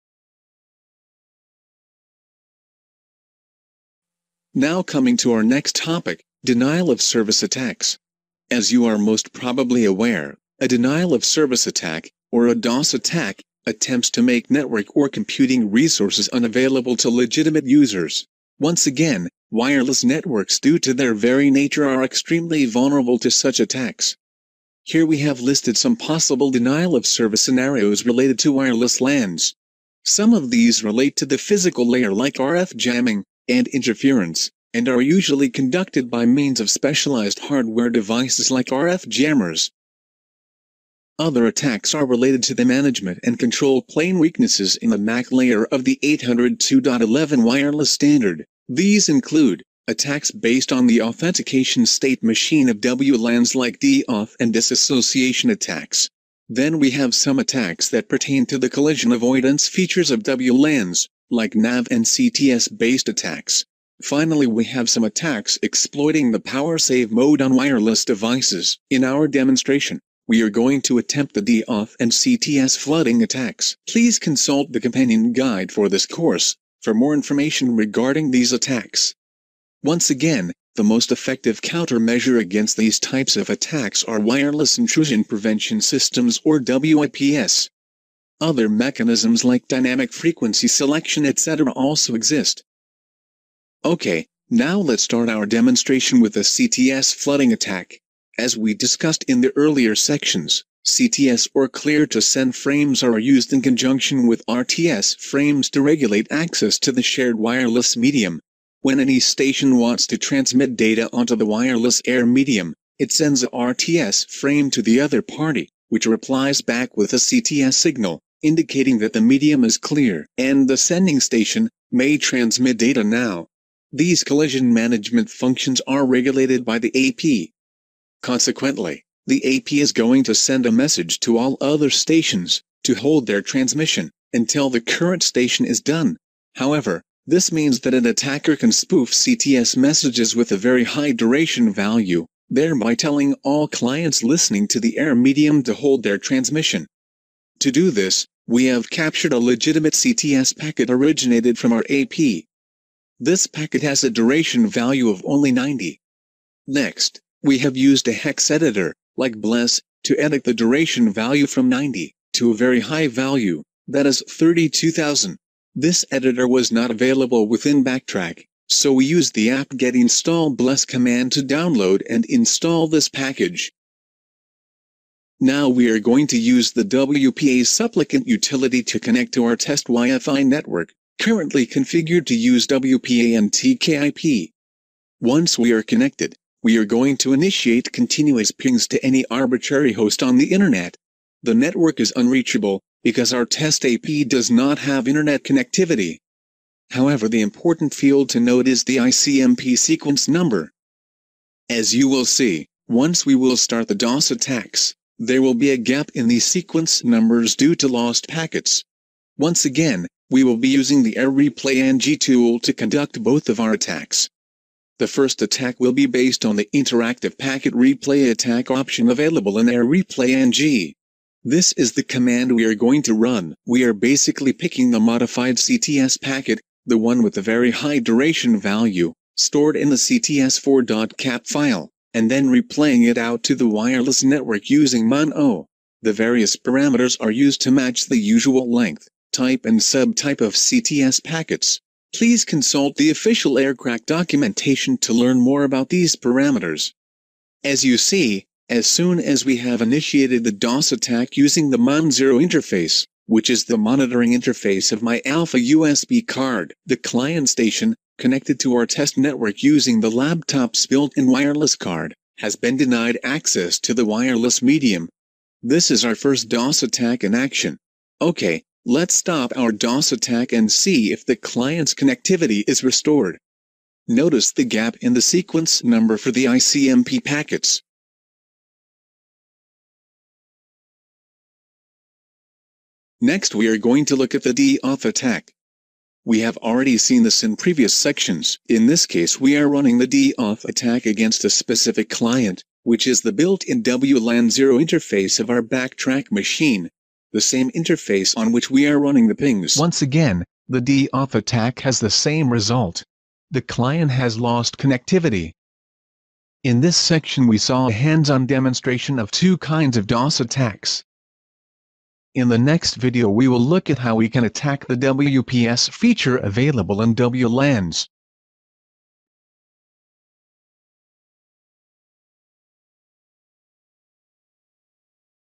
BackTrack 5 Wireless Penetration Testing Tutorial: Denial-of-service (DoS) Attacks | packtpub.com
Simulate a Denial-of-service (DoS) attack that is meant to shut down a machine or network.
o A practical demonstration of DoS attacks against Wireless LAN infrastructure.
o Discussing various types of wireless Denial-of-Service attacks. Connecting your BackTrack machine's built-in Wi-Fi interface to our lab AP. Then starting a continuous ping to any arbitrary host.
o Executing the Deauthentication attack by targeting a specific client, once again using the aireplay-ng utility.